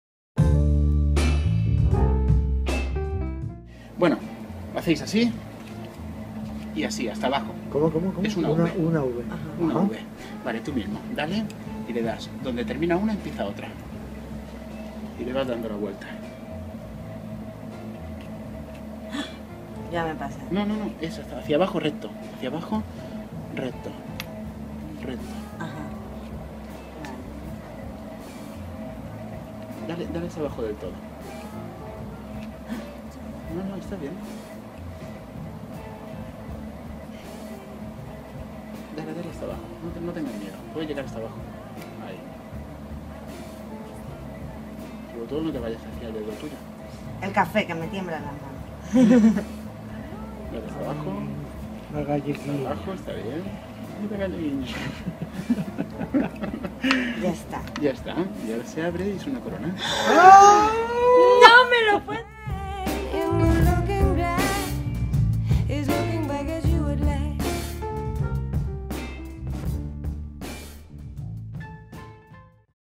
Curso de decoración. "Corona" con Nacho Viñas. Koketo
Curso de decoración con Nacho Viñas, autor del libro "Decoración práctica para hostelería". Aprendiendo a tallar frutas, verduras y hortalizas. Un gran profesional, gran maestro y excelente persona.